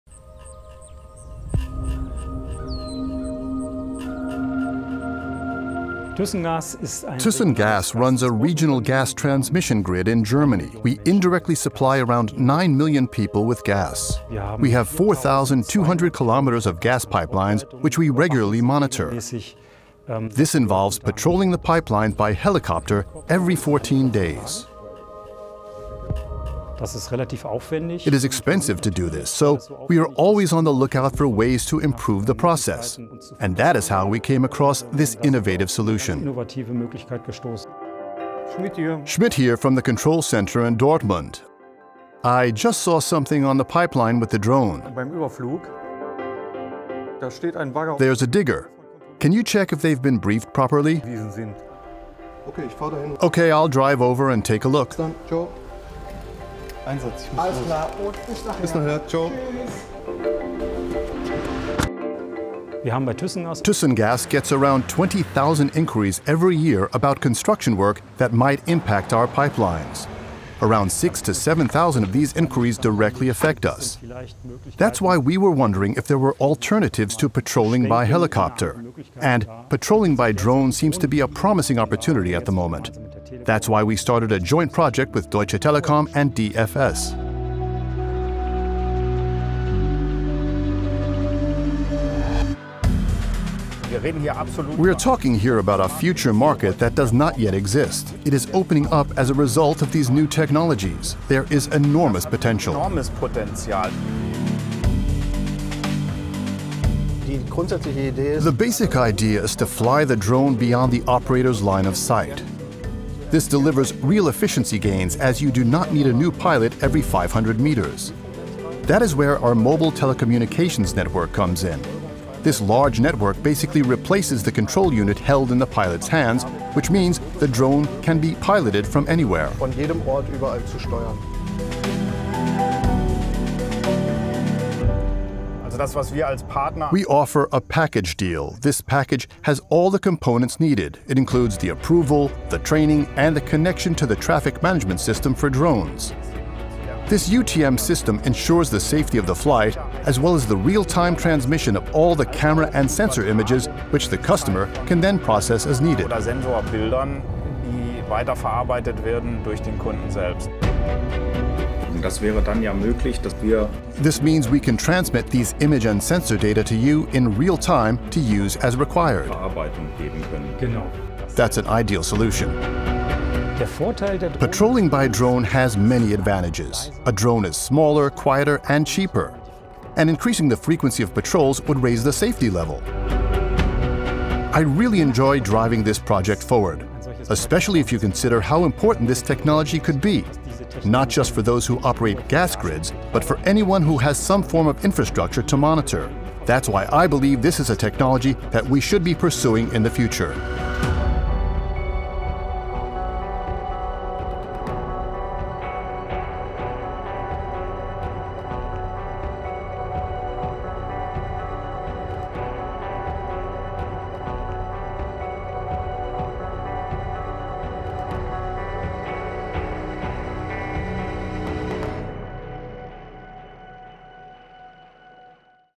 Drones connected: Aerial Patrol of Pipelines
DFS Deutsche Flugsicherung and Deutsche Telekom are testing together with Thyssengas drone operations for pipeline inspections.